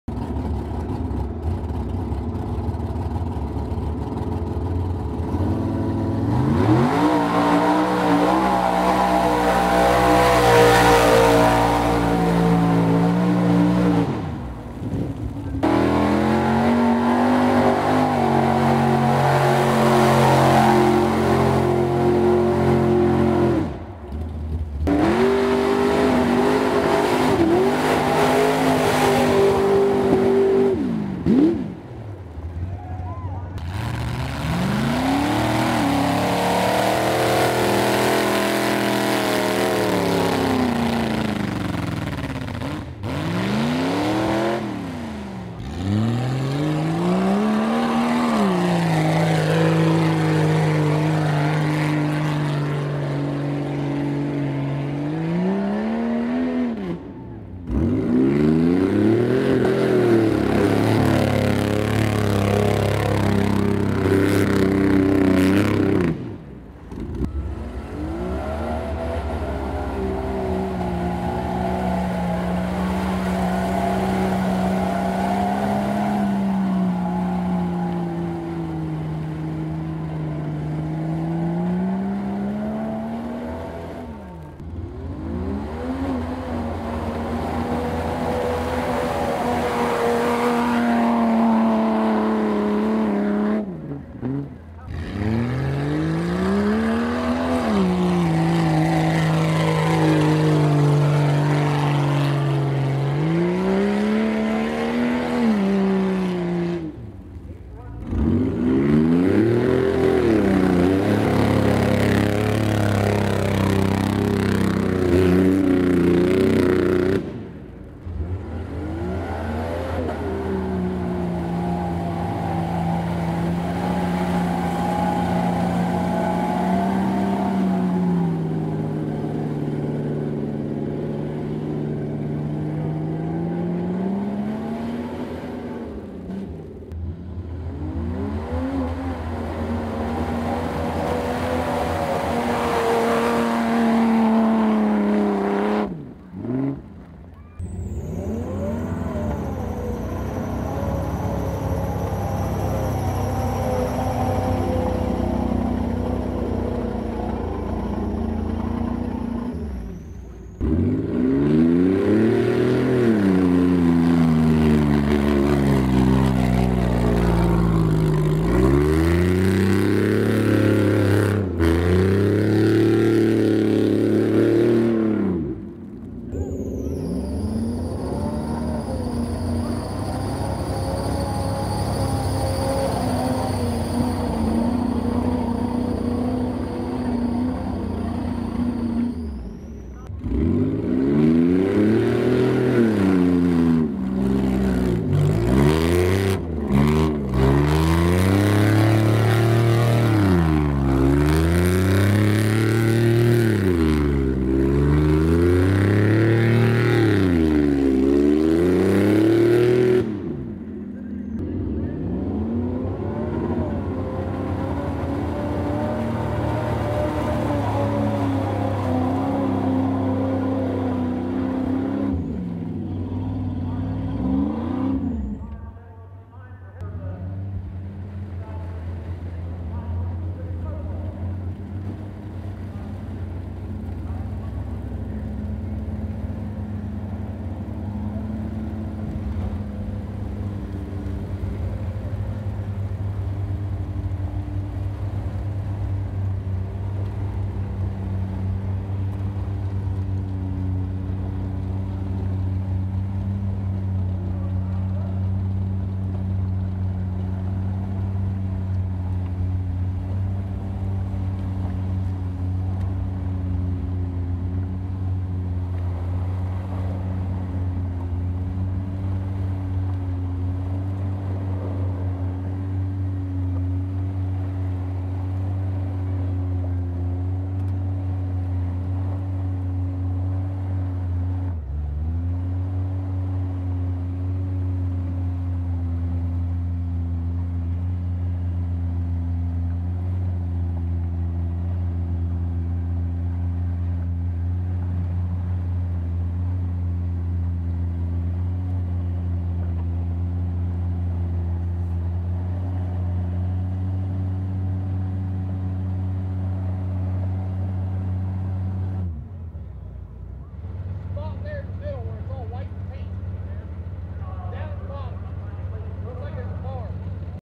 Deep Pit #3 Vics Mud Bog October 13, 2018
October 13, 2018 was a cold day in St Louisville, OH but the mud slinging and good times went on at Vic’s Mud Bog regardless.  The kids power wheels and mud foot run was held in between the big boys slinging the mud in the speed pit and then the deep pit.  The great people at Vic’s even provided a pork covered dish dinner for everyone.  Hillbilly Proud loves to go to Vic’s for the great hospitality and great people.